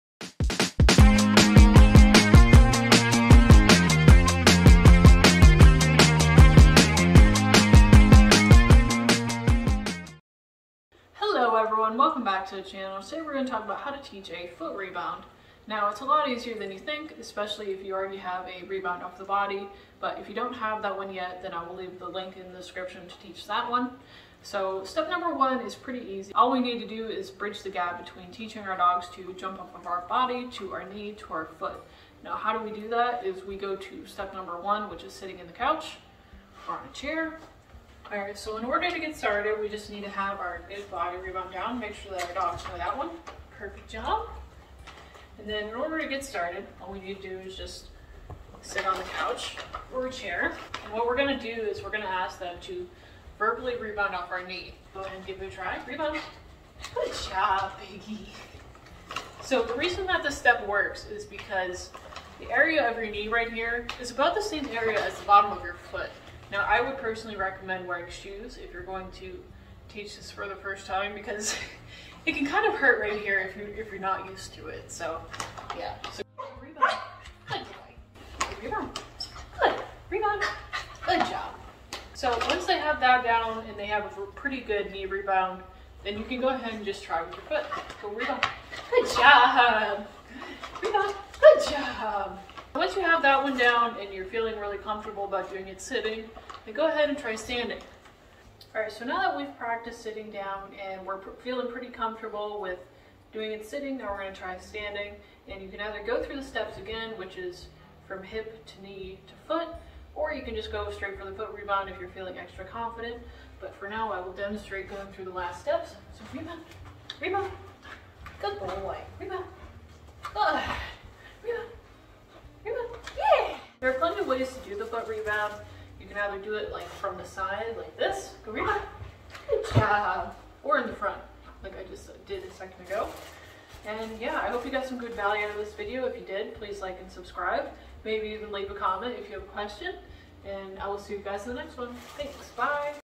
Learn FOOT REBOUND dog trick!
In this video I break down teaching the foot rebound in a way that makes the most sense from body rebound to knee to foot.

Teach a dog rebound, teach a dog to rebound off your foot, foot rebound, foot dog jump, jump off feet dog, puppy